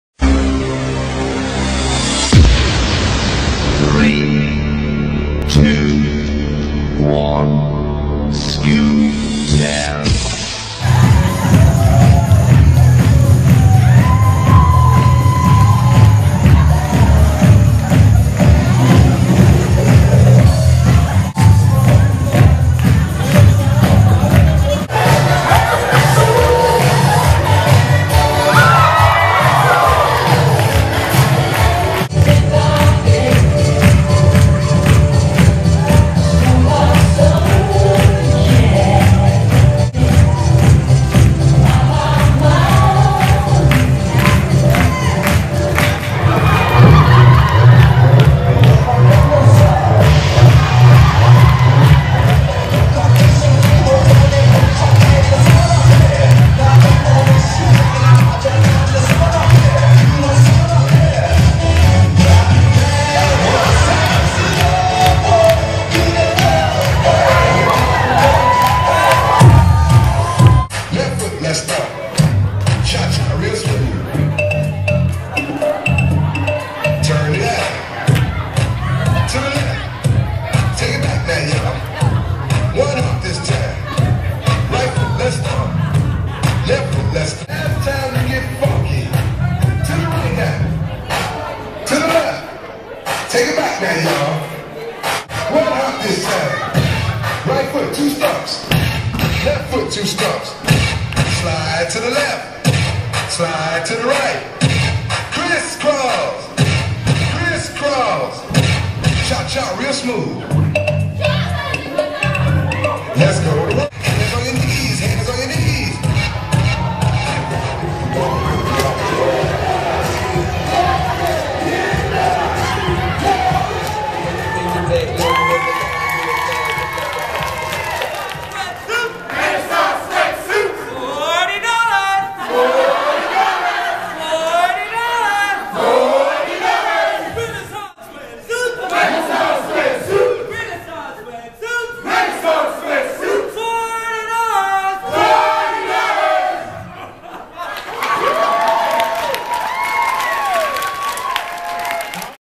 BRCPS PD Dec 2012
BRCPS Fashion Show during the professional development meeting on December 7, 2012